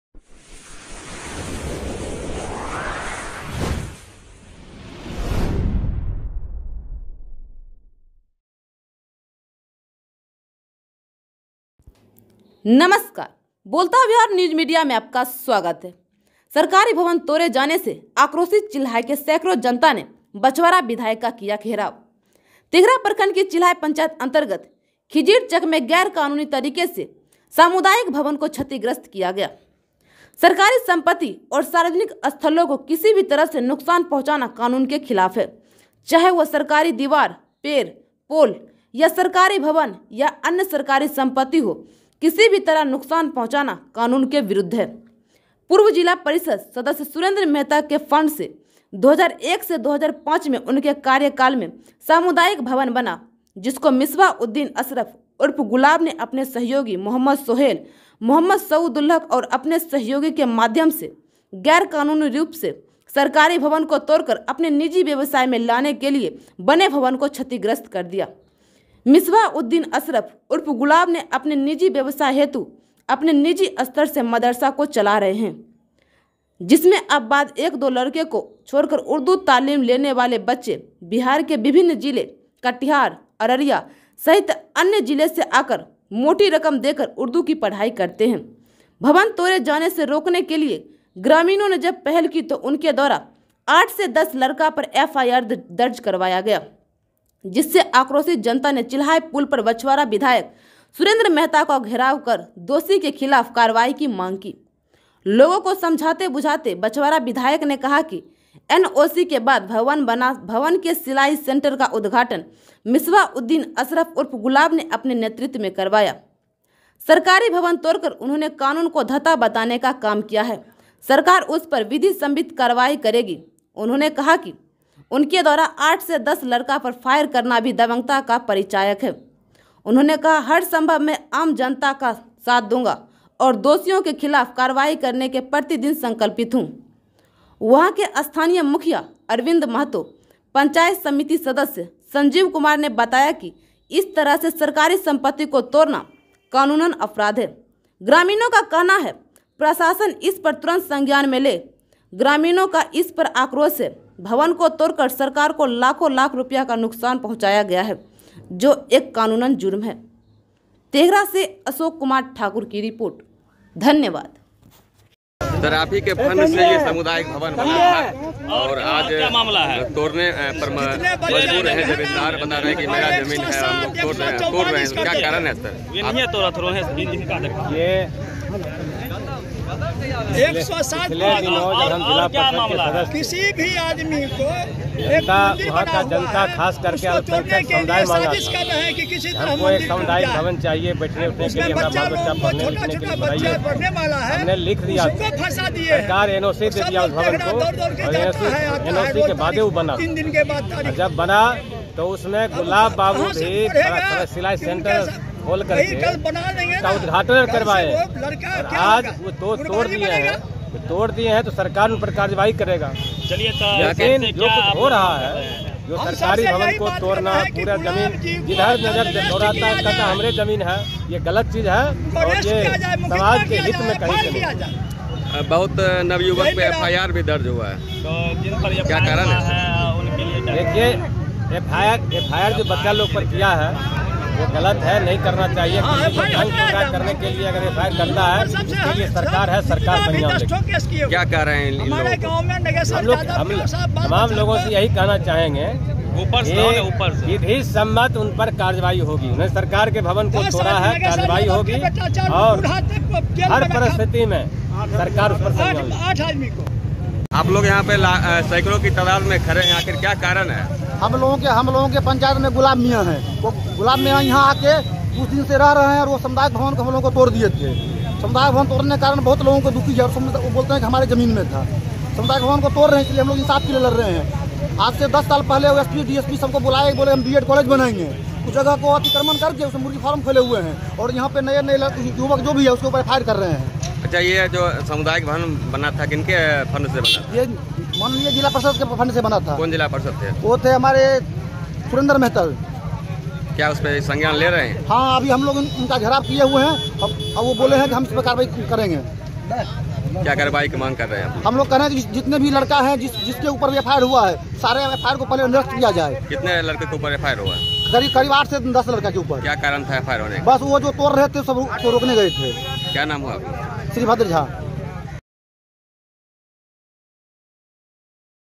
भवन तोड़े जाने से आक्रोशित चिल्हाय के जनता ने बछवारा विधायक का किया घेराव
सरकारी भवन तोड़े जाने  से आक्रोशित चिल्हाय के सैकड़ों जनता ने बछवारा विधायक का किया घेराव
अशोक कुमार ठाकुर, तेघरा (बेगूसराय)
तेघरा प्रखंड के चिल्हाय पंचायत अंतर्गत खिजीर चक में  गैर कानूनी तरीके से सामुदायिक भवन को क्षतिग्रस्त किया गया।
सरकारी संपत्ति और सार्वजनिक स्थलों को किसी भी तरह से नुकसान पहुंचाना कानून के खिलाफ है।चाहे वह सरकारी दीवार , पेड़, पोल, या सरकारी भवन या अन्य सरकारी संपत्ति हो किसी भी तरह नुकसान पहुंचाना कानून के विरुद्ध है । पूर्व जिला परिषद सदस्य सुरेंद्र मेहता के फंड से (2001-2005) में  उनके कार्यकाल में सामुदायिक भवन बना जिसको मिसवा उद्दीन अशरफ उर्फ गुलाब  ने अपने सहयोगी मोहम्मद सोहेल , मोहम्मद सऊदुलहक और अन्य सहयोगी के माध्यम से गैर कानूनी रूप से से सरकारी भवन को तोड़कर अपने निजी व्यवसाय में लाने के लिए बने भवन  को क्षतिग्रस्त कर दिया ।मिस्वाह उद्दीन अशरफ उर्फ गुलाब ने अपने निजी व्यवसाय हेतु अपने निजी स्तर से मदरसा को चला रहे हैं जिसमें अपवाद एक दो लड़के को छोड़कर उर्दू तालीम लेने वाले बच्चे बिहार के विभिन्न जिले कटिहार , अररिया सहित अन्य जिले से आकर  मोटी रकम देकर उर्दू की पढ़ाई करते हैं ।  भवन तोड़े जाने से रोकने के लिए ग्रामीणों ने जब पहल की तो उनके द्वारा 8 से 10 लड़का पर एफ आई आर भी दर्ज करवाया गया । जिससे आक्रोशित जनता ने चिल्हाय पुल पर बछवारा विधायक सुरेंद्र मेहता का घेराव कर दोषी के खिलाफ कार्रवाई की मांग की। लोगों को समझाते बुझाते बछवारा विधायक ने कहा कि एनओसी के बाद भवन बना भवन में सिलाई सेंटर का उद्घाटन मिसवा उद्दीन अशरफ उर्फ गुलाब ने अपने नेतृत्व में करवाया। सरकारी भवन तोड़कर उन्होंने कानून को धता बताने का काम किया है सरकार उस पर विधि संबित कार्रवाई करेगी। उन्होंने कहा कि उनके द्वारा 8 से 10 लड़का पर फायर करना भी दबंगता का परिचायक है। उन्होंने कहा हर संभव मै आम ग्रामीण का साथ दूंगा और दोषियों के खिलाफ कार्रवाई करवाने के प्रति दिन संकल्पित हूं।   वहां के स्थानीय मुखिया अरविंद महतो , पंचायत समिति सदस्य संजीव कुमार ने बताया कि इस तरह से सरकारी संपत्ति को तोरना कानूनी अपराध है। ग्रामीणों का कहना है प्रशासन इस पर तुरंत संज्ञान में ले ग्रामीणों का इस पर आक्रोश है भवन को तोड़कर सरकार को लाखों लाख रुपया का नुकसान पहुंचाया गया है जो एक कानूनन जुर्म है।